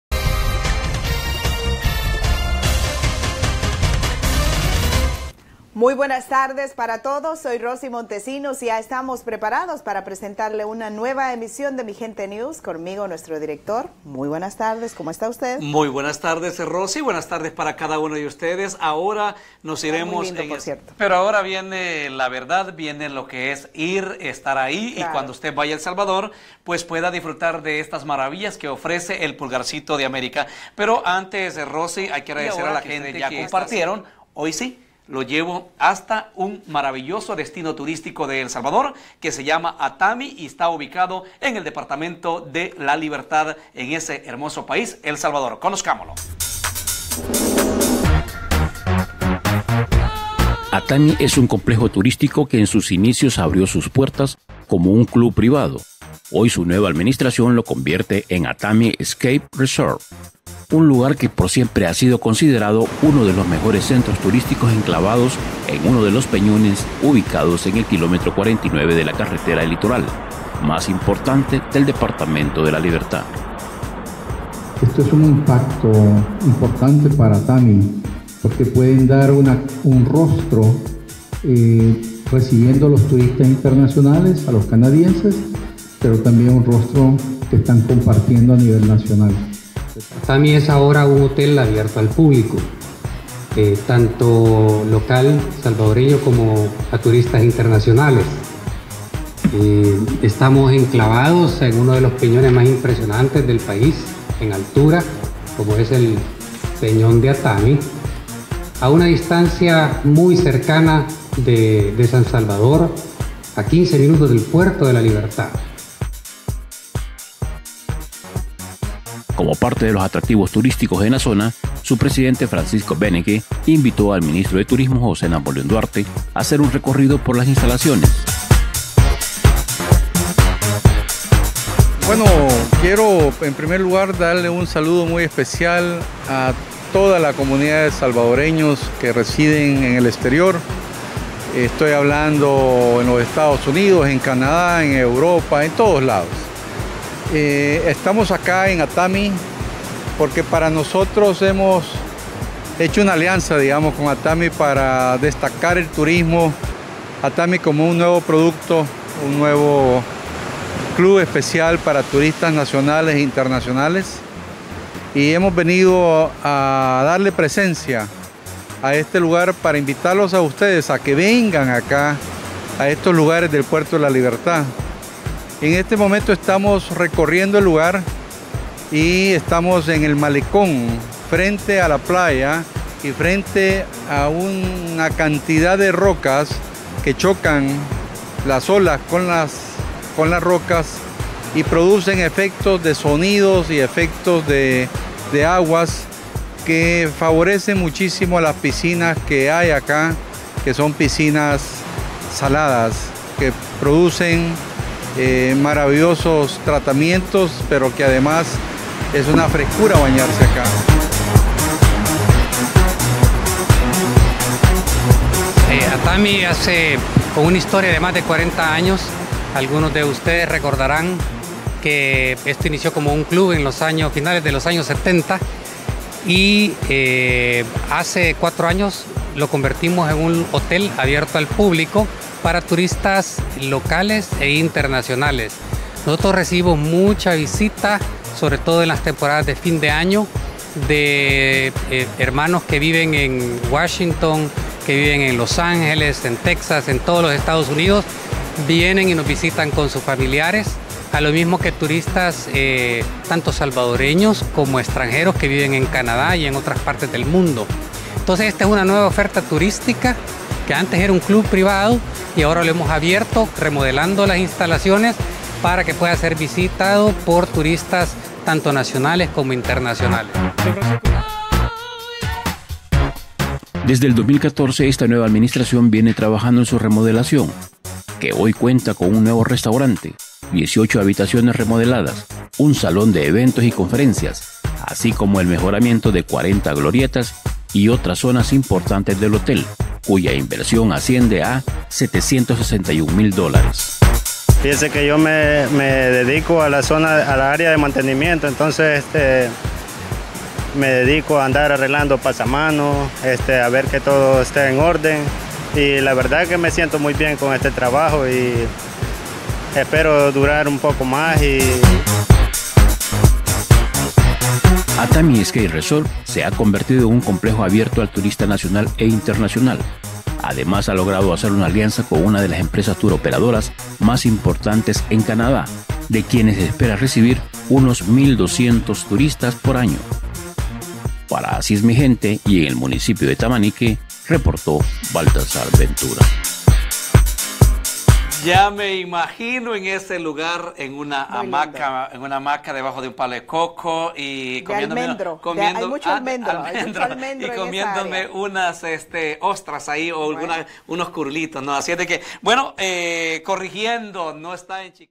ATAMI ESCAPE RESORT
ATAMI ES UN COMPLEJO TURÍSTICO QUE EN SUS INICIOS ABRIÓ SUS PUERTAS COMO UN CLUB PRIVADO. HOY SU NUEVA ADMINISTRACIÓN LO CONVIERTE EN ATAMI ESCAPE RESORT.

UN LUGAR QUE POR SIEMPRE HA SIDO CONSIDERADO UNO DE LOS MEJORES CENTROS TURÍSTICOS ENCLAVADO EN UNO DE LOS PEÑONES UBICADOS EN EL KILÓMETRO 49 DE LA CARRETERA EL LITORAL MAS IMPORTANTES DEL DEPARTAMENTO DE LA LIBERTAD. AGOSTO 2017